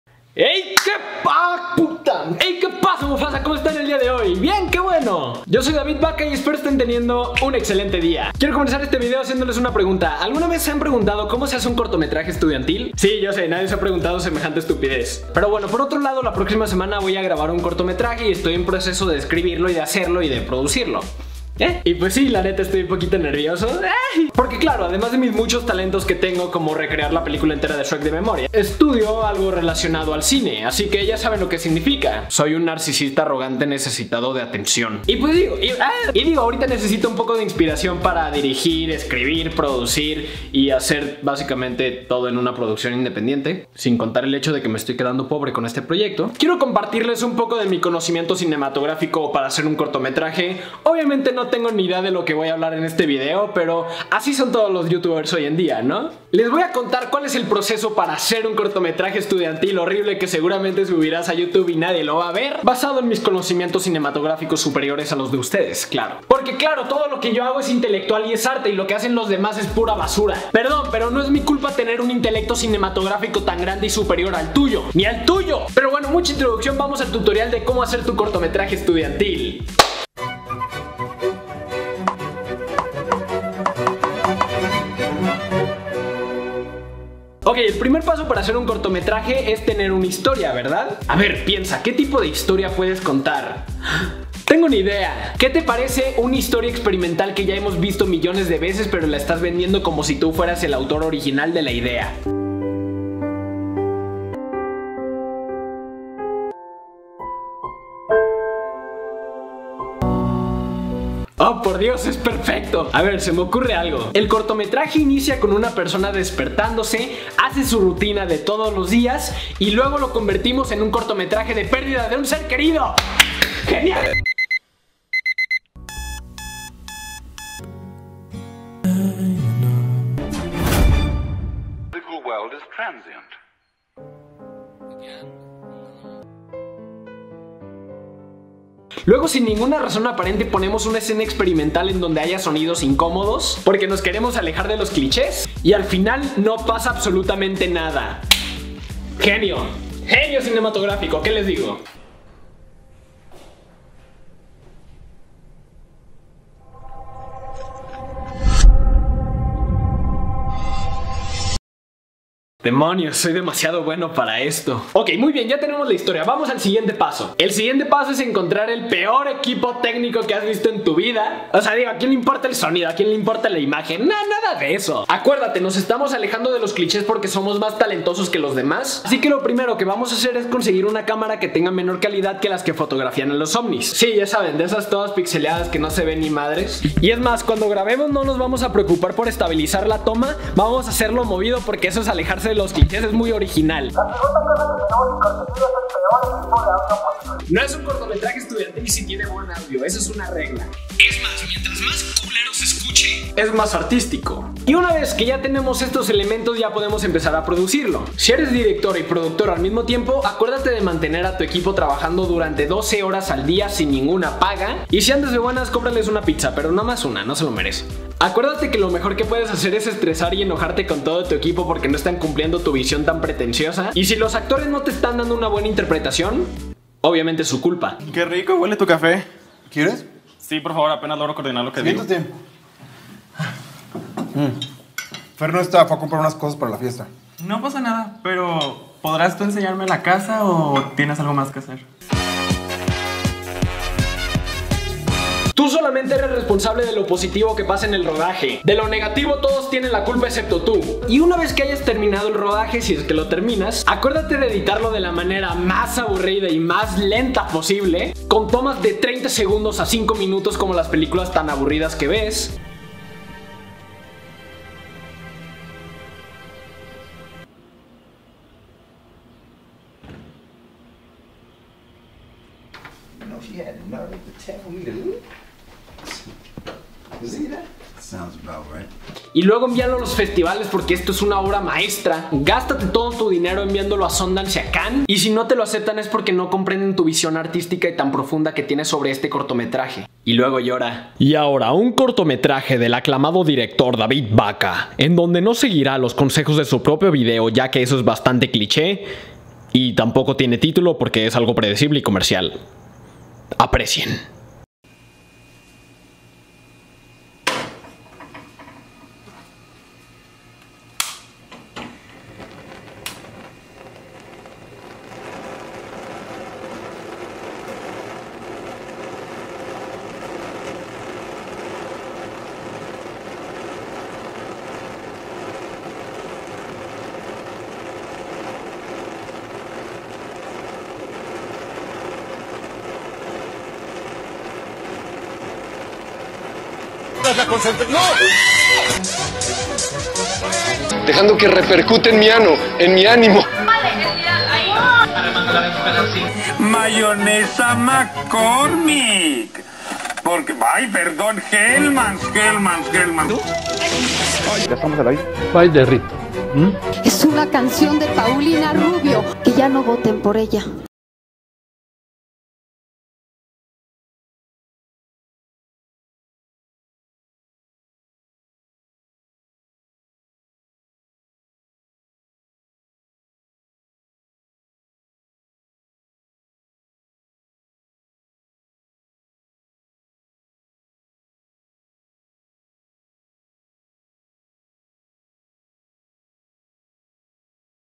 Cómo hacer un CORTO ESTUDIANTIL - Tutorial
En este nuevo video tutorial les comparto mi gran conocimiento cinematográfico para hacer un cortometraje estudiantil para que ustedes los simples mortales sean lo que es el verdadero arte y me aprecien como el ser superior que soy.